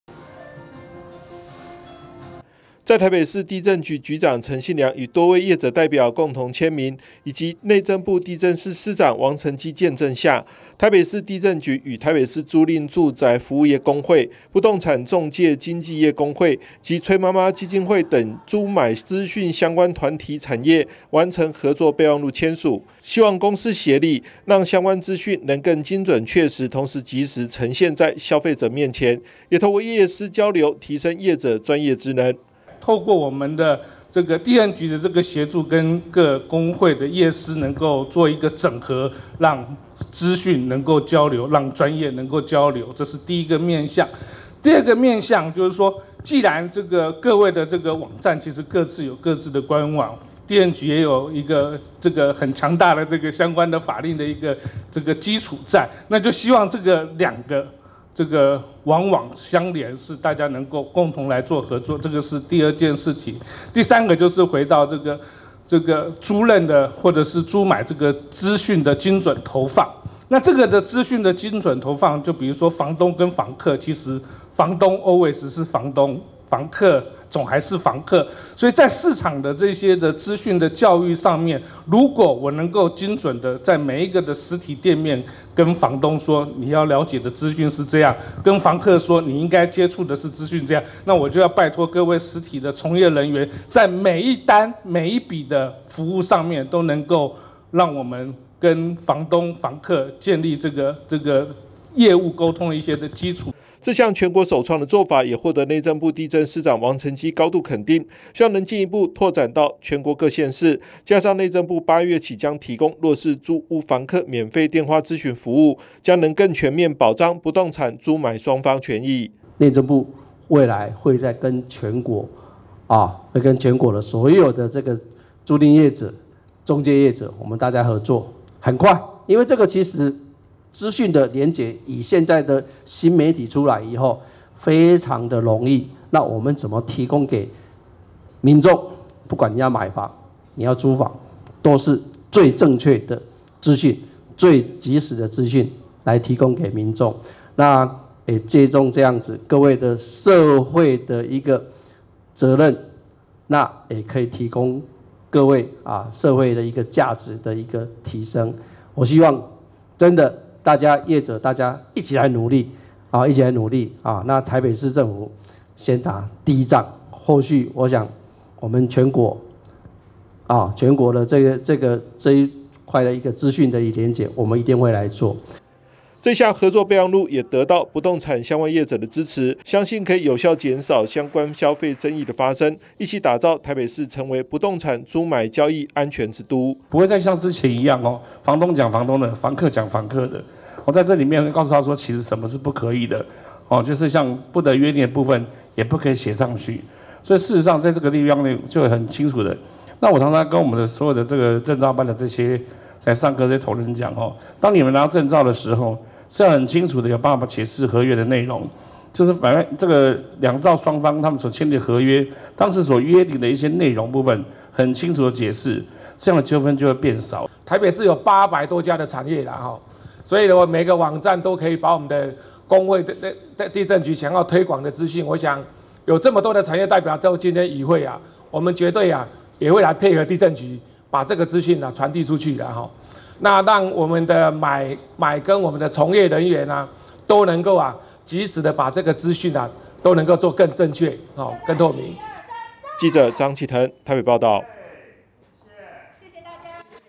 房價 台北|租屋 買房|實價登錄 交易安全|仲介 租賃|2024 中嘉新聞|打造不動產租買安全網 北市府與業者簽MOU
打造不動產租買安全網 北市府創全國之先與業者簽訂MOU 資訊共享人員培訓

為了推廣不動產租買透明，提供民眾即時正確的租買資訊，從源頭減少消費爭議，台北市地政局與相關租賃資訊產業、團體共同簽署合作備忘錄，透過共享行銷通路及教育訓練資源，擴大資訊推廣，與台北市八百多家產業公私協力共同打造台北市成為不動產交易安全之都。

在台北市地政局長陳信良與多位業者代表共同簽名以及內政部地政司司長王成機見證下，台北市地政局與台北市租賃住宅服務業公會、不動產仲介經紀業公會及崔媽媽基金會等租買資訊相關團體產業完成合作備忘錄簽署，希望公私協力讓相關資訊能更精準確實同時即時呈現在消費者面前，也透過業師交流提升業者專業知能。

這項全國首創的作法也獲得內政部地政司長王成機高度肯定，希望能進一步拓展到全國各縣市，加上內政部八月起將提供弱勢租屋房客免費電話諮詢服務，將能更全面保障不動產租買雙方。

這項合作備忘錄也得到不動產相關業者的支持，相信可以有效減少相關消費爭議的發生，一起打造台北市成為不動產租買交易安全之都。